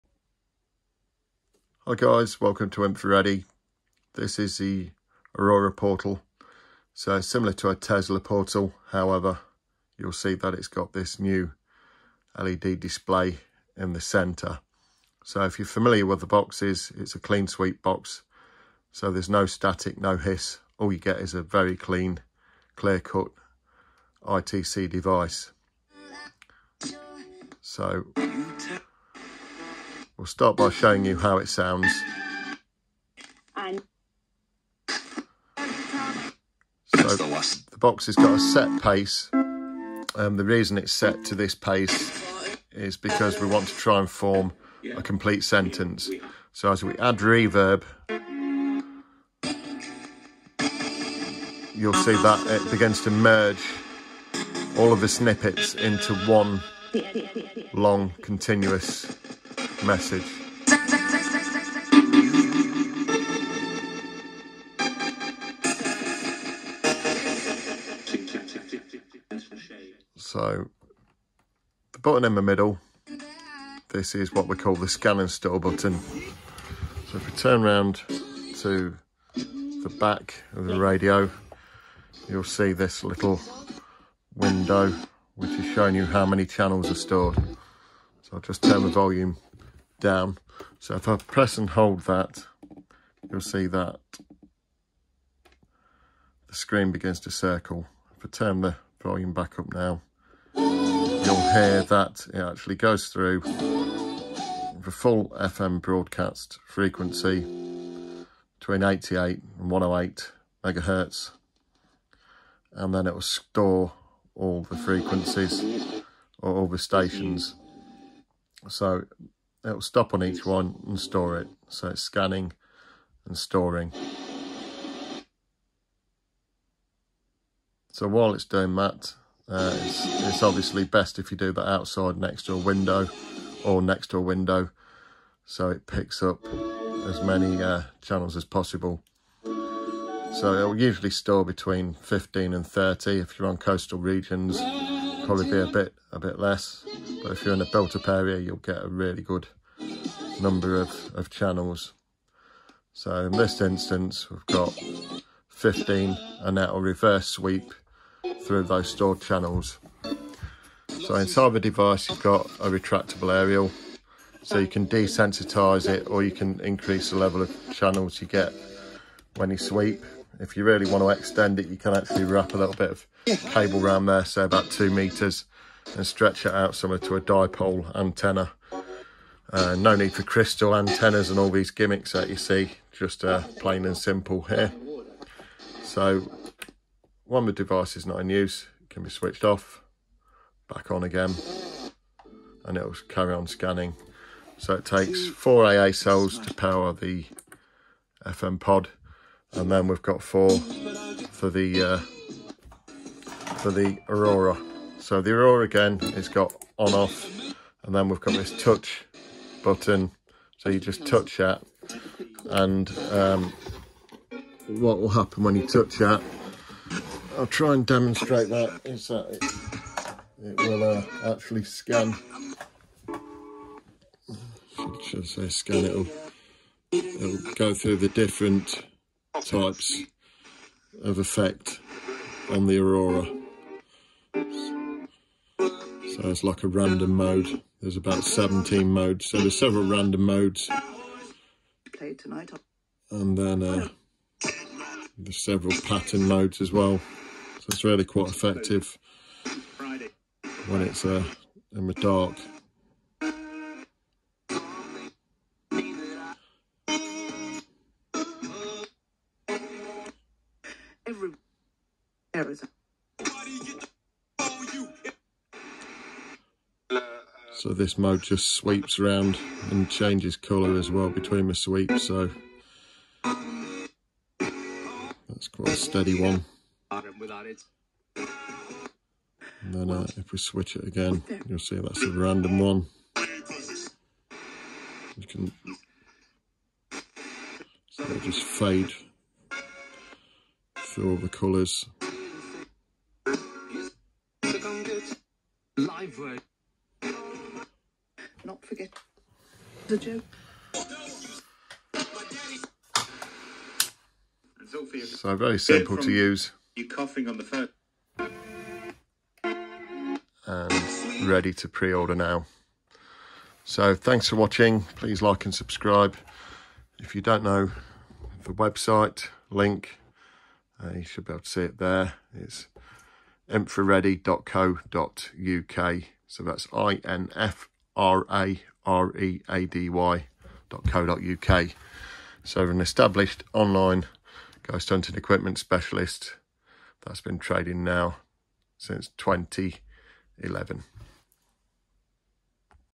Sweep radio ghost box infraportal
Aurora led display infraportal ghost box sweep radio spirit portal. Clean sweep scan and store ITC for clear communication.